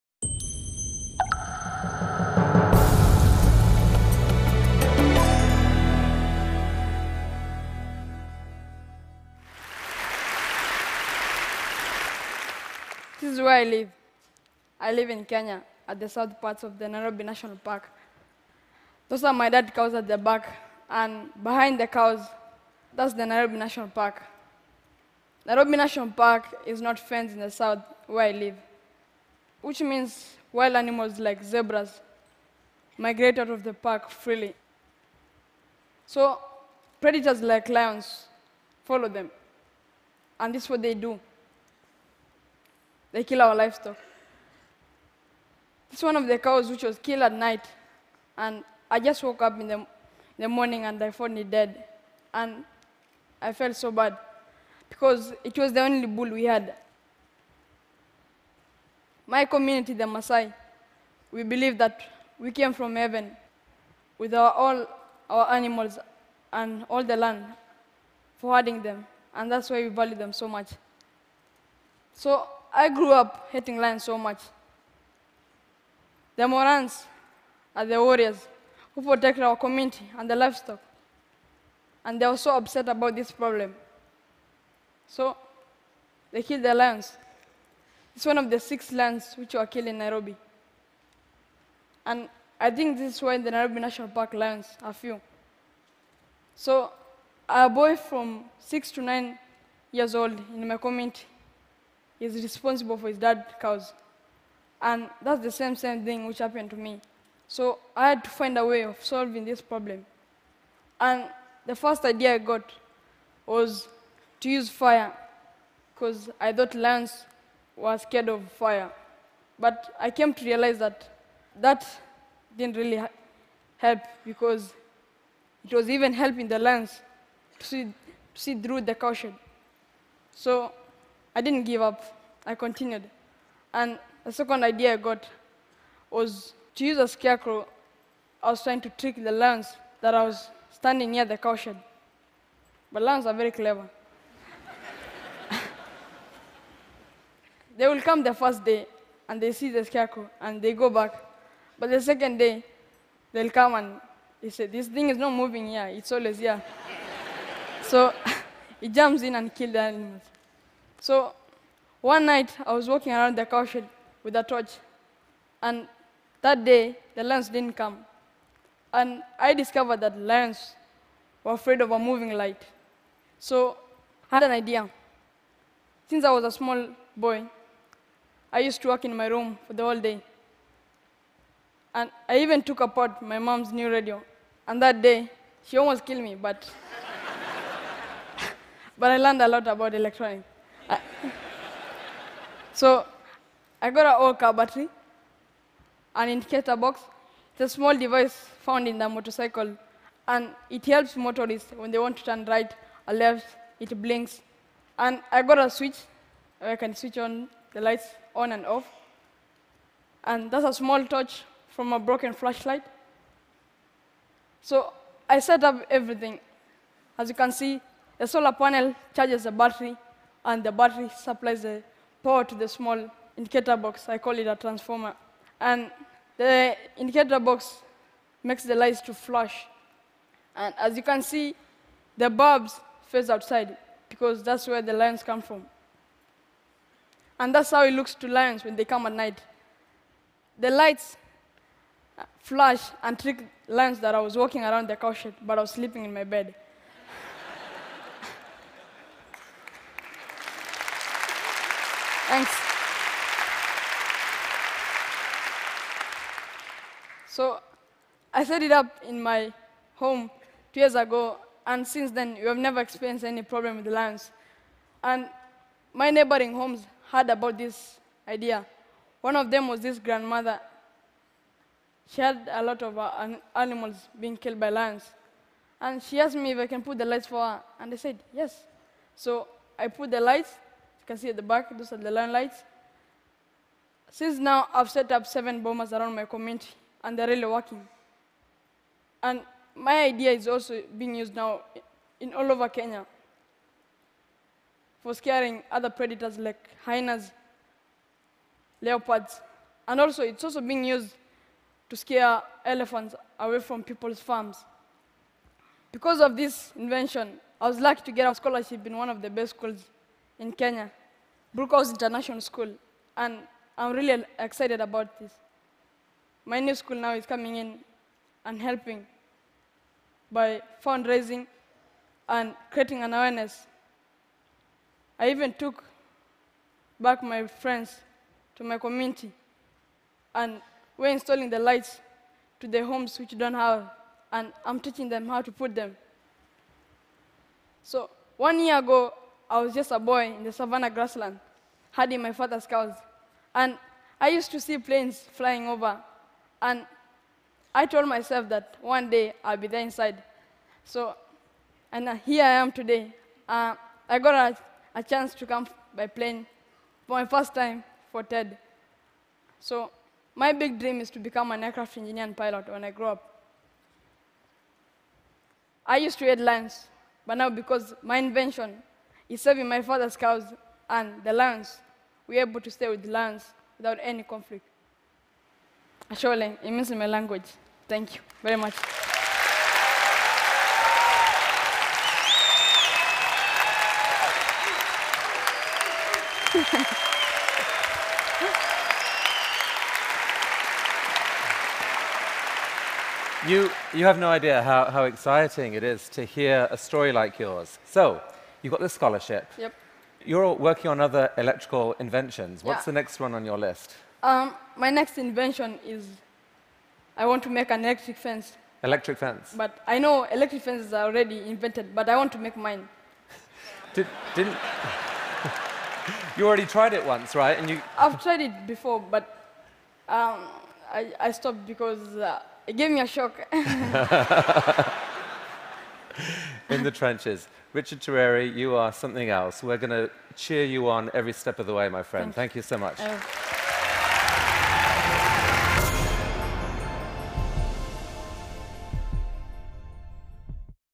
Richard Turere: My invention that made peace with lions
In the Masai community where 13-year-old Richard Turere lives, cattle are all-important. But lion attacks were growing more frequent. In this short, inspiring talk, the young inventor shares the solar-powered solution he designed to safely scare the lions away.